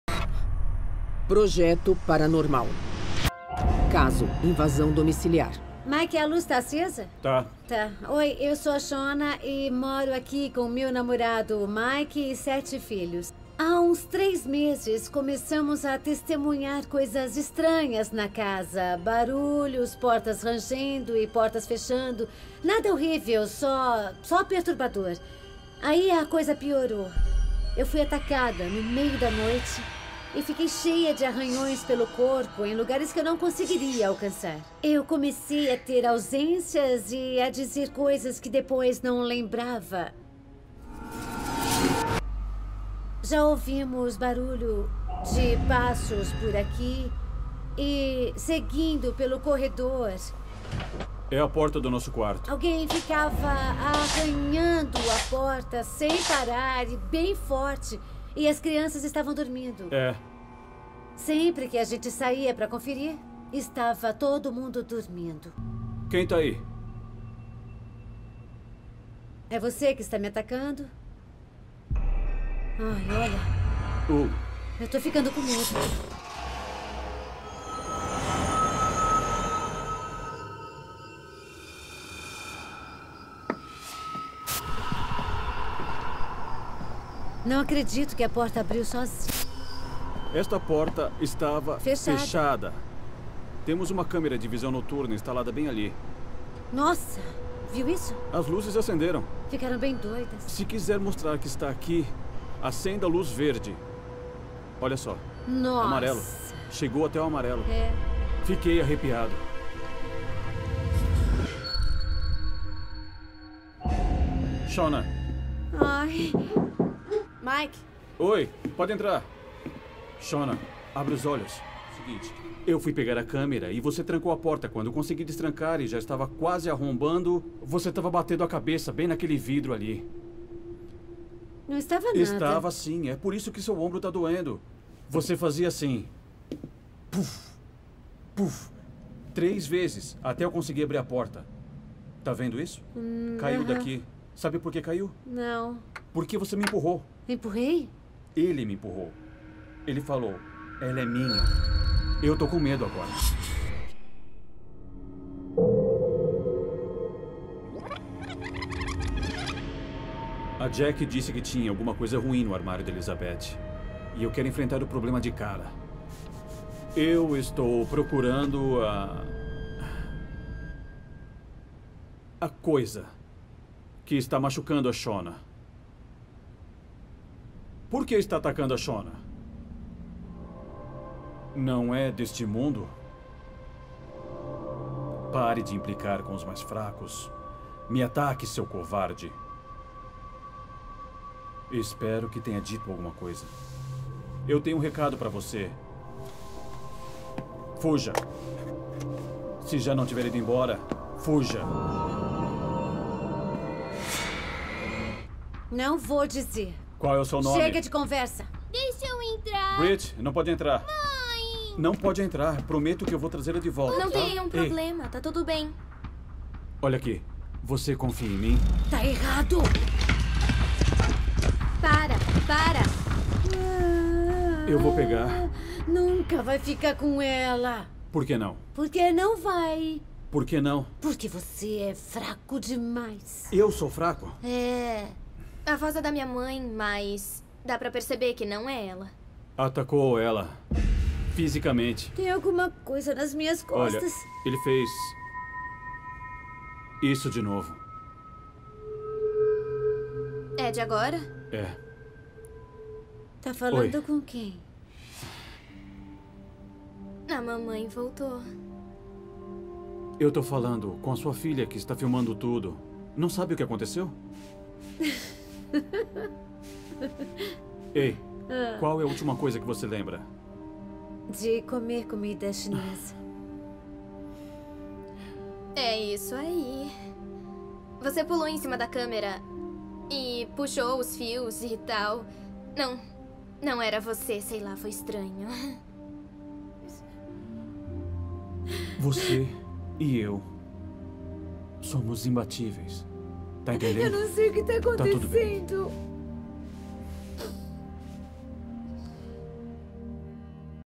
Ataque do Além
Uma força sobrenatural ataca uma mulher em sua própria casa.